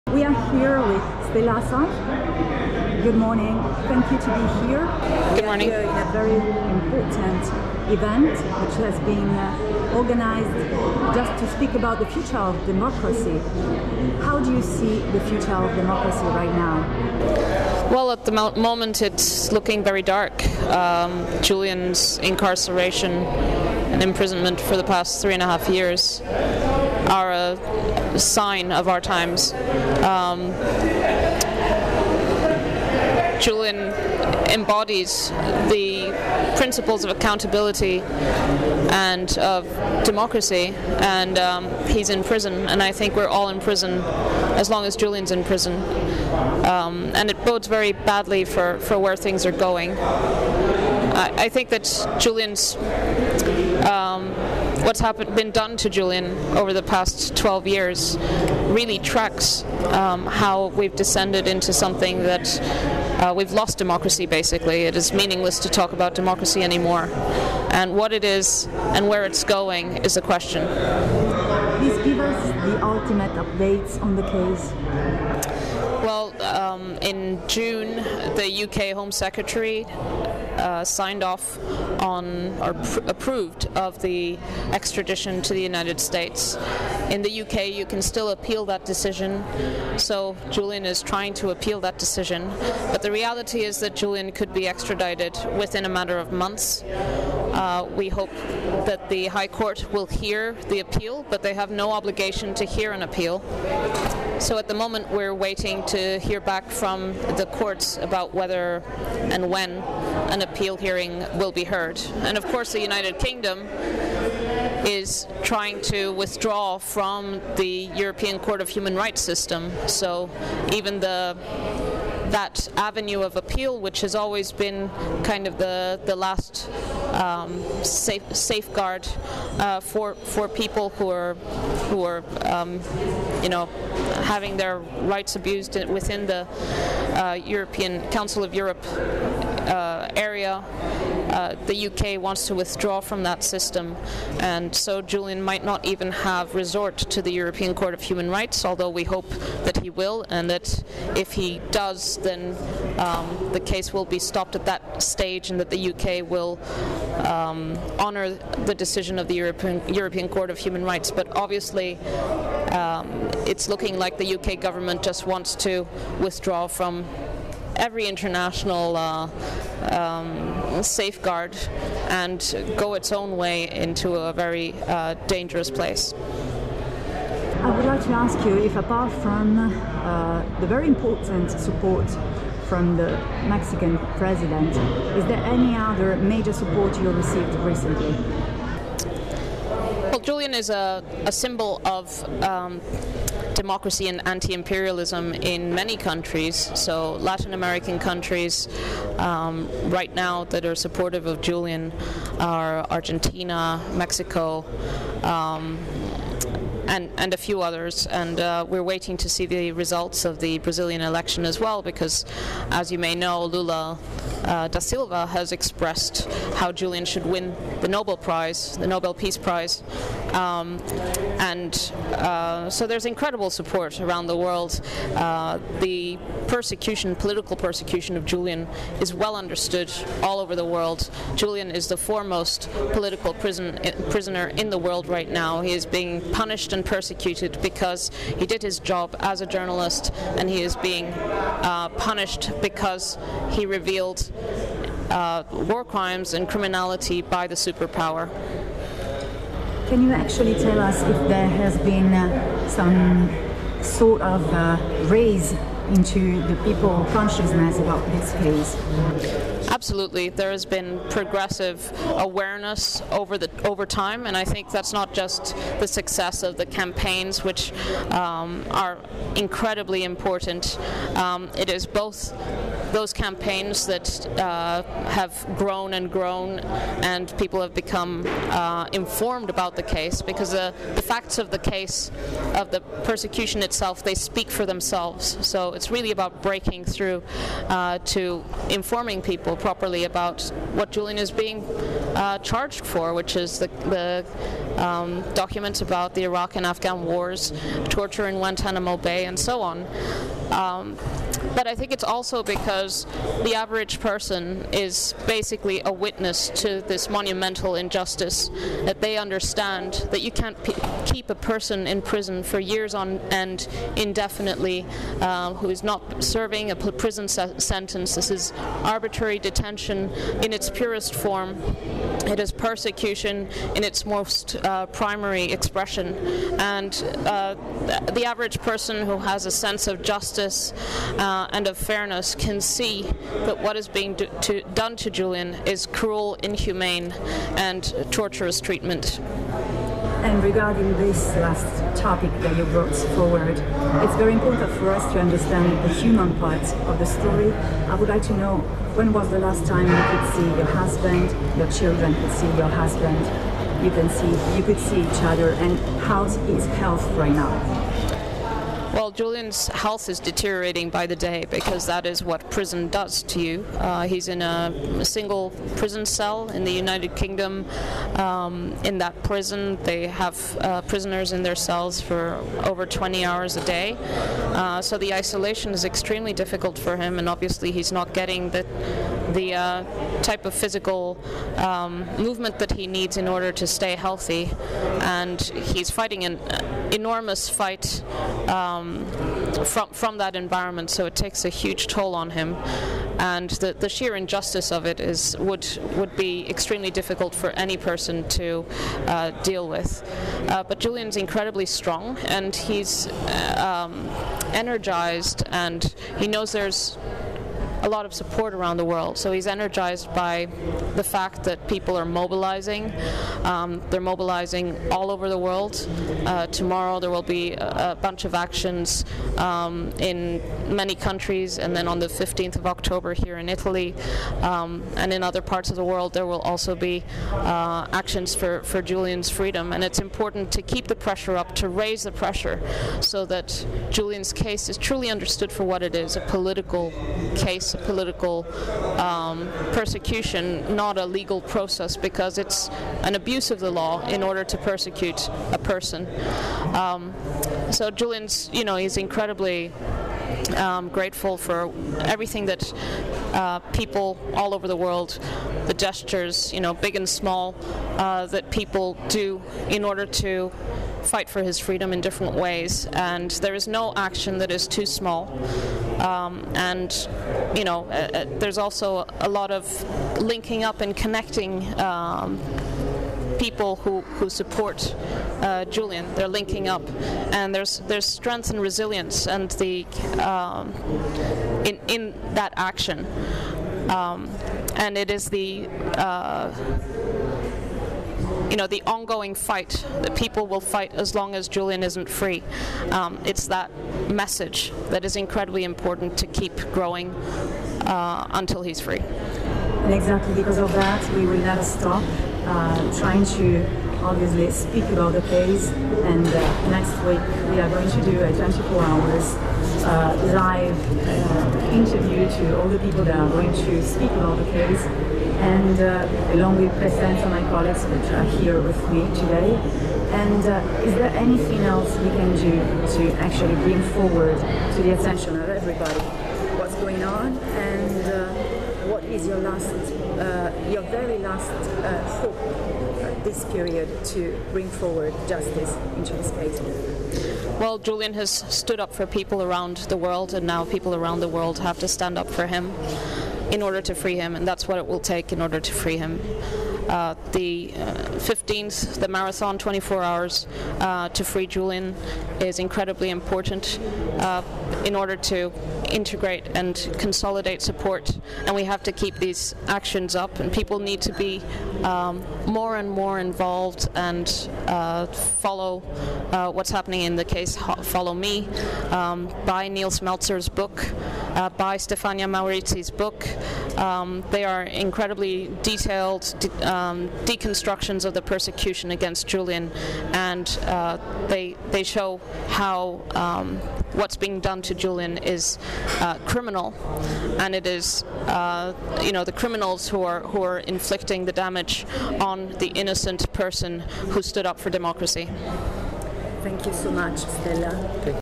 Interview with Stella Assange, October 7, 2022, Milan, Italy
Wired Next Fest - The Future of Democracy, Fabbrica del Vapore, Milan 07/10/2022.

Veronica Tarozzi interviews Stella Assange about the latest events in the case of Julian Assange and the present and future actions in support of his release.

Technical support from Renato Napoli and Maurizio Torti.